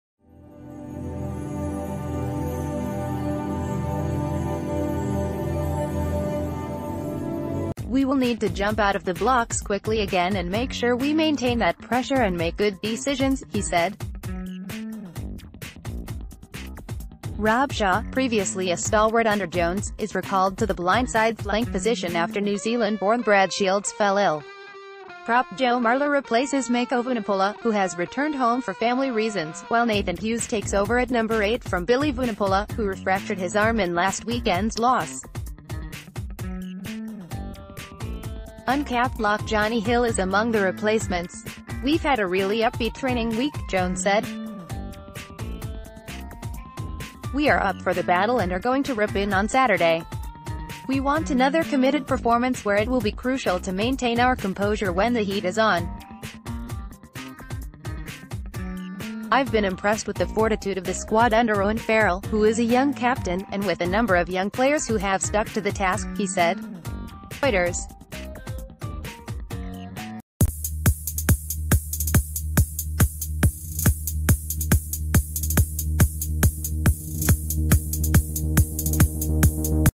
Breaking News | England’s Danny Cipriani gets his first start in 10 years
"We will need to jump out of the blocks quickly again and make sure we maintain that pressure and make good decisions," he said. Robshaw, previously a stalwart under Jones, is recalled to the blindside flank position after New Zealand-born Brad Shields fell ill. Prop Joe Marler replaces Mako Vunipola, who has returned home for family reasons, while Nathan Hughes takes over at No8 from Billy Vunipola, who refractured his arm in last weekend’s loss. Uncapped lock Jonny Hill is among the replacemen...